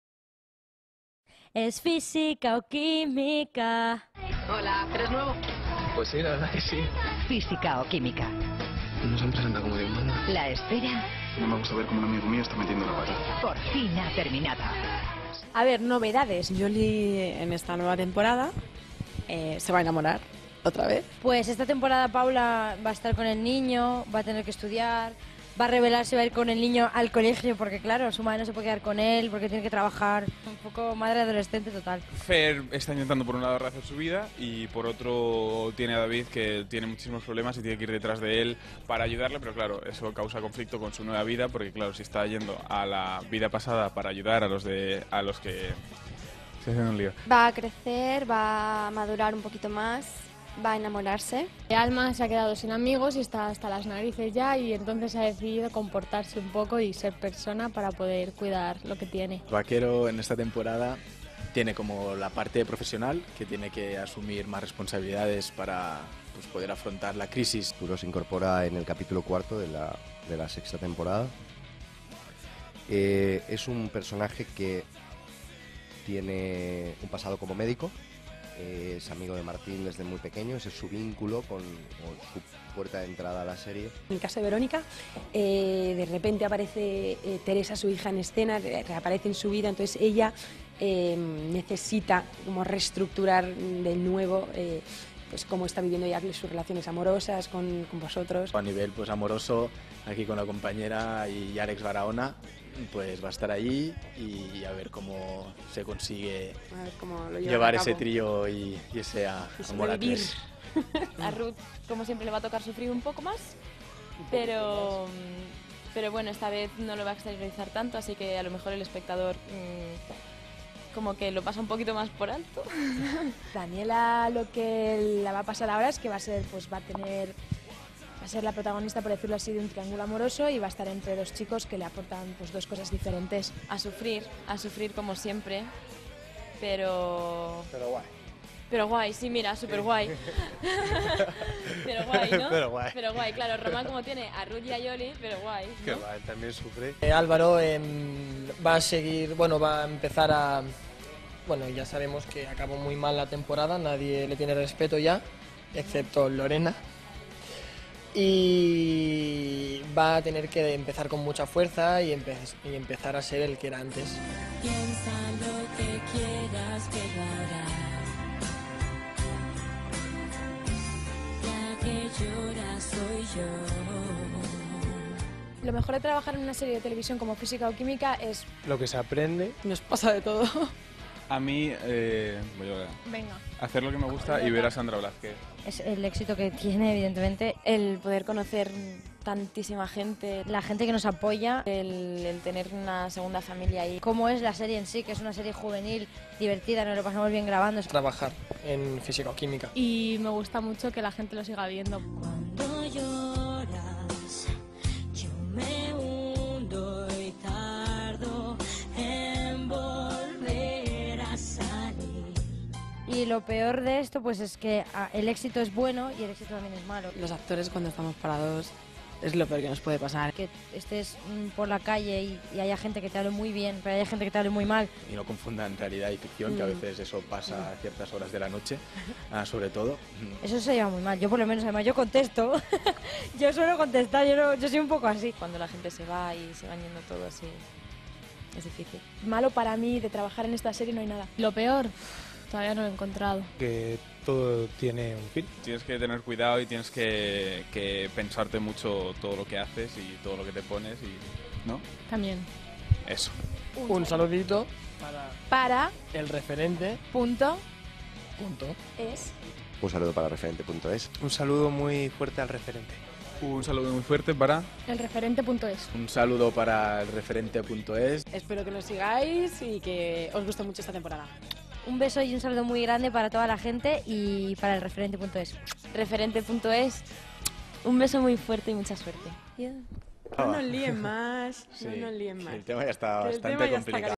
Entrevista ElReferente - Lo nuevo de la 6ºTemporada
El Referente ha estado con los actores de FoQ para saber un poquito más sobre la nueva etapa en la que algunos abandonarán la serie y caras nuevas se incorporan al elenco habitual.